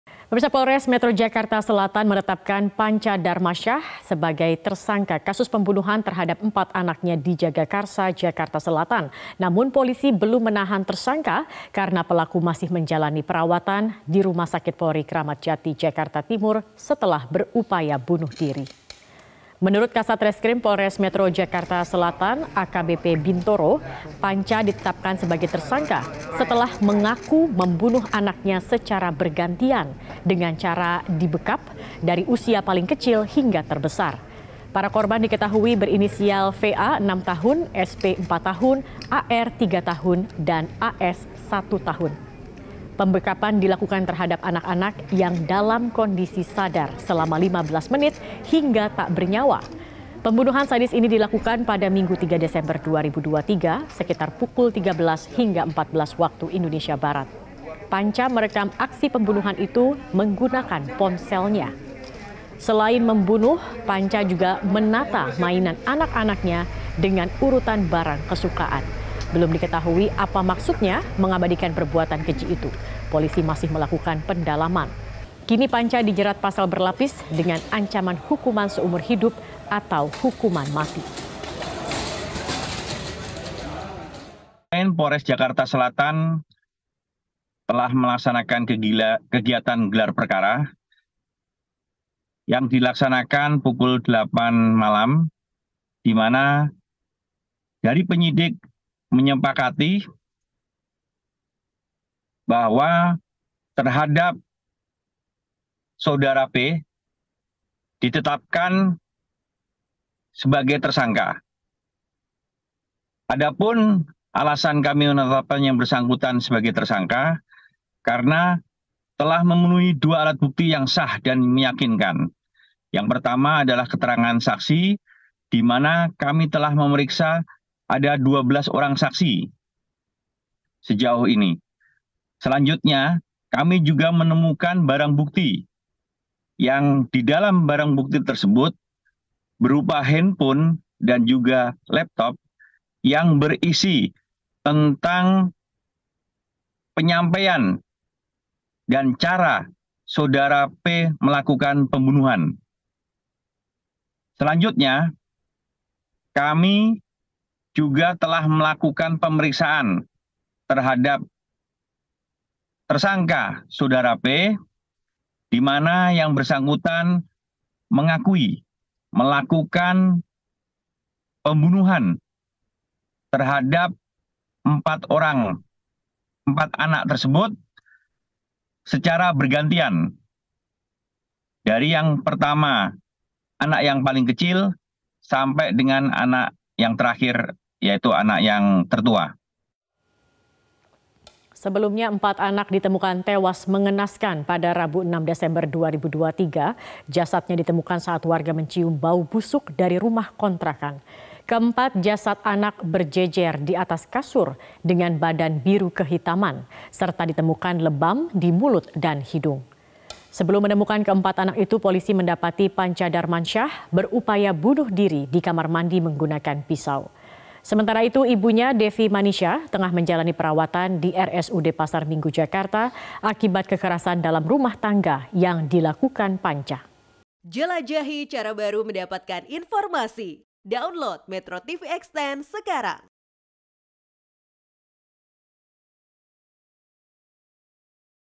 Pelaku Rekam Aksi P3m8vnvh4n Dengan Ponsel
MetroTV, Tim Forensik rumah sakit Polri Kramat Jati sudah selesai melakukan autopsi terhadap 4 anak korban p3mbunuh4n. Meski demikian pihaknya masih menunggu hasil pemeriksaan laboratorium patologi.